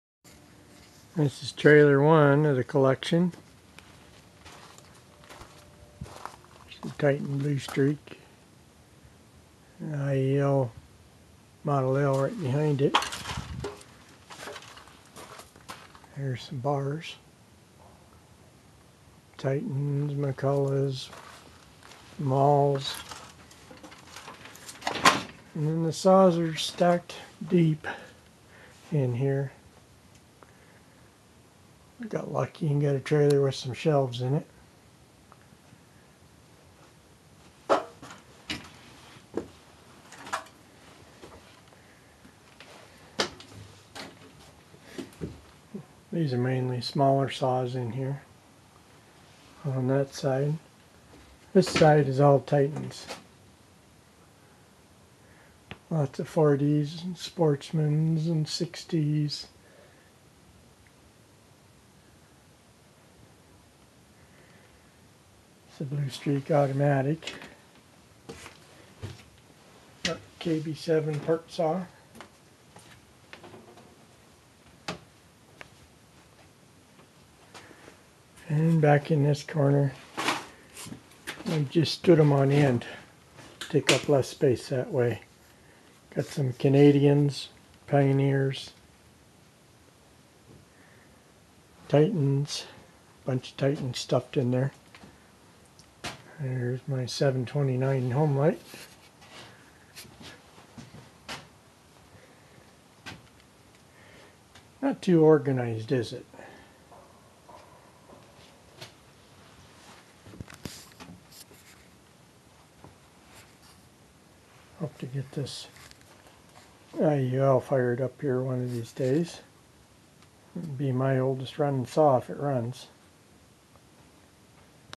chain saw collection 1
trailer loaded with old chainsaws